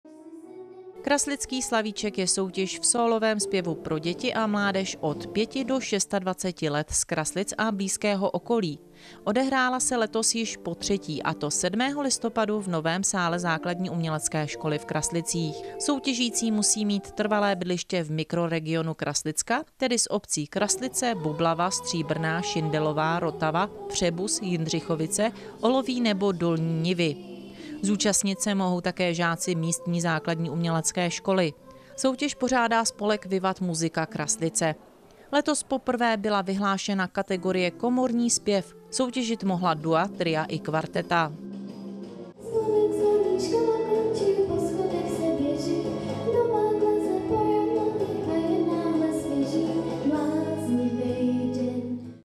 Kraslice: Ve Slavíčku letos poprvé soutěžily i skupiny (TV Západ)
Kraslický Slavíček je soutěž v sólovém zpěvu pro děti a mládež od 5 do 26 let z Kraslic a blízkého okolí. Odehrála se letos již potřetí, a to 7. listopadu v novém sále ZUŠ v Kraslicích

Soutěžící musí mít trvalé bydliště v mikroregionu Kraslicka, tedy z obcí Kraslice, Bublava, Stříbrná, Šindelová, Rotava, Přebuz, Jindřichovice, Oloví nebo Dolní Nivy. Zúčastnit se mohou také žáci místní Základní umělecké školy. Soutěž pořádá spolek Vivat Musica Kraslice.

Letos poprvé byla vyhlášená kategorie "komorní zpěv". Soutěžit mohla dua, tria i kvarteta.